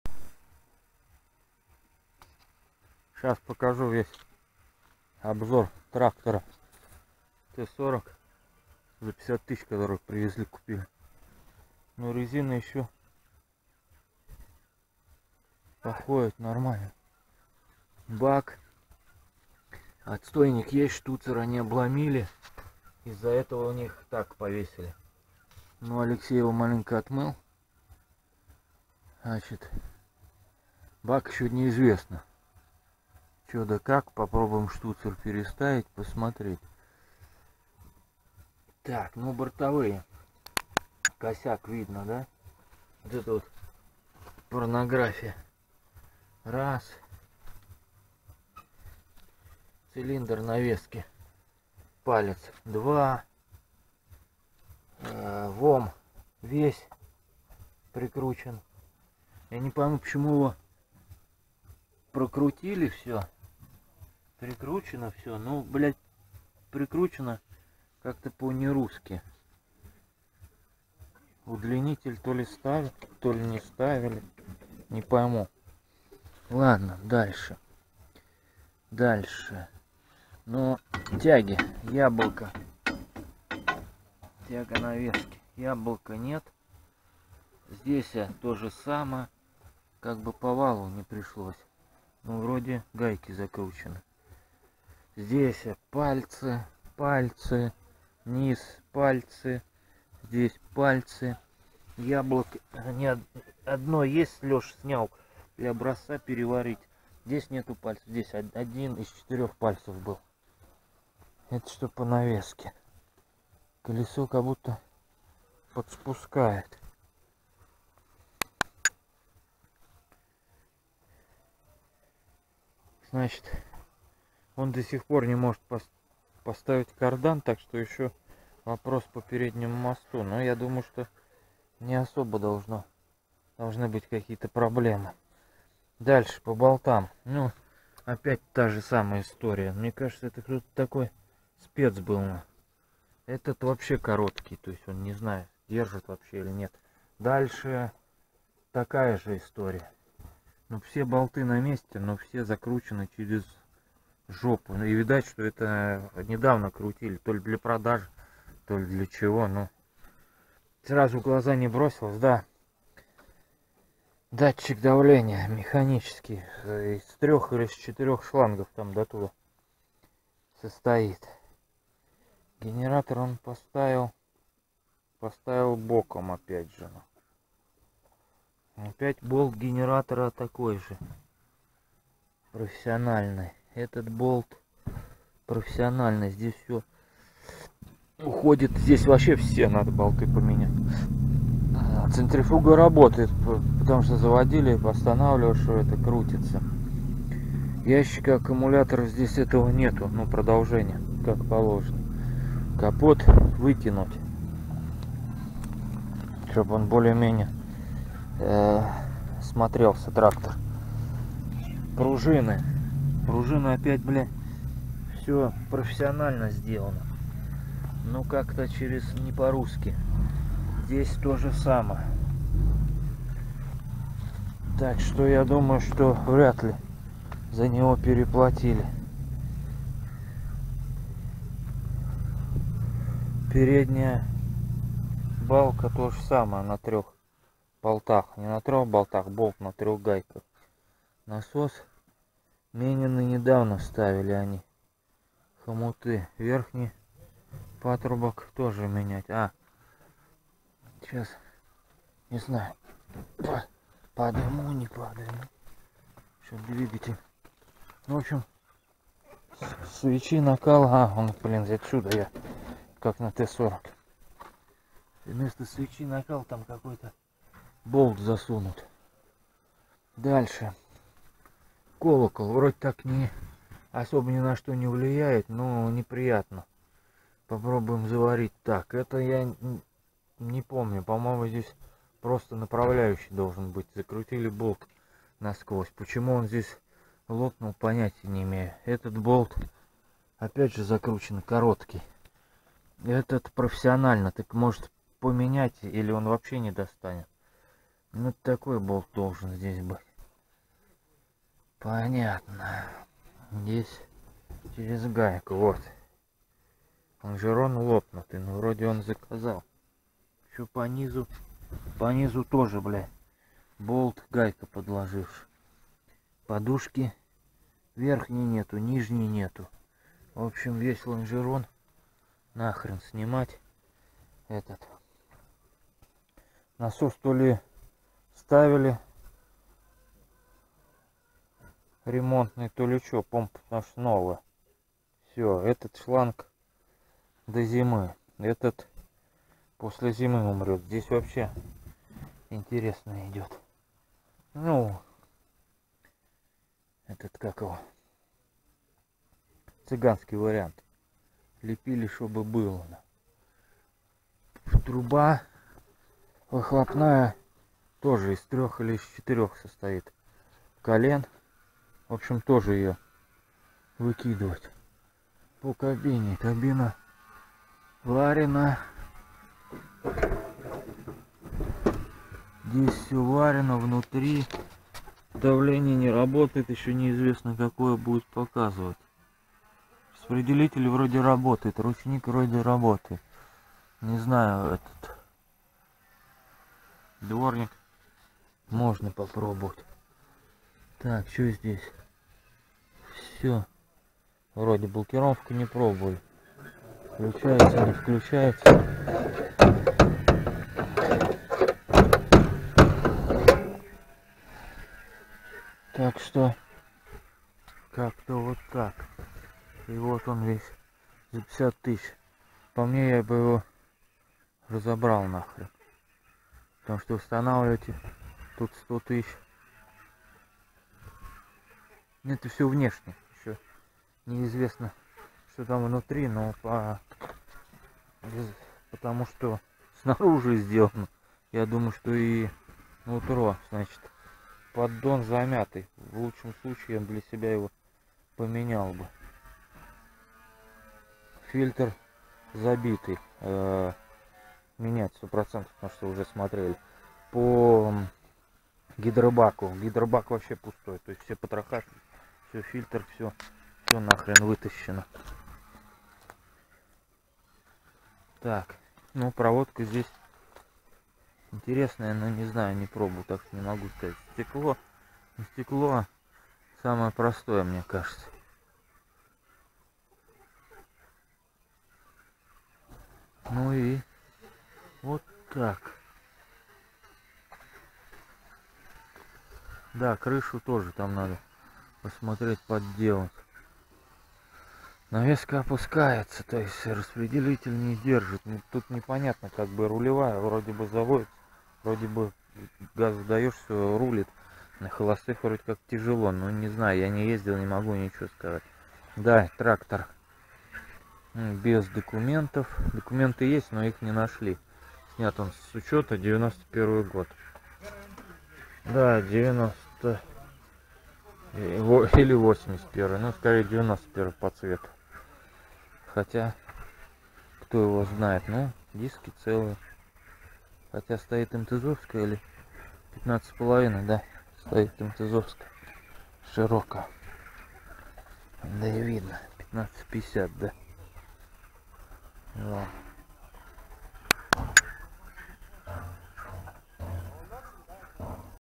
Обзор трактора Т40 за 50 000 руб!ЧТО КУПИЛИ ВОЛОСЫ СТОЯТ ДЫБОМ!
разочарование от покупки подобных тракторов!
Да Да- несомненно он стоит своих денег!
но все таки хотелось большего!
но вышло как вышло!
теперь предстоит работа по ремонту!!